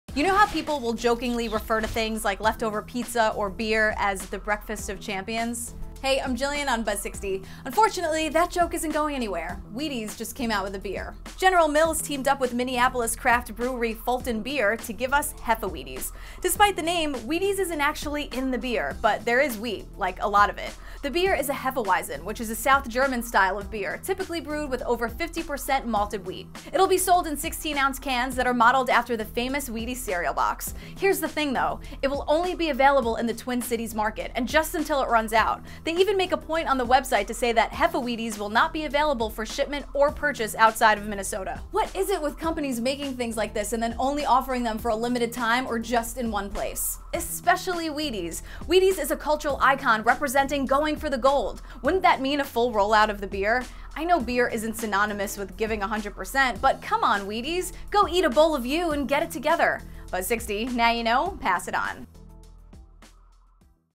Wheaties is coming out with a beer
General Mills teamed up with Minneapolis craft brewery Fulton Beer to give us HefeWheaties. Gillian Pensavalle (@GillianWithaG) has more.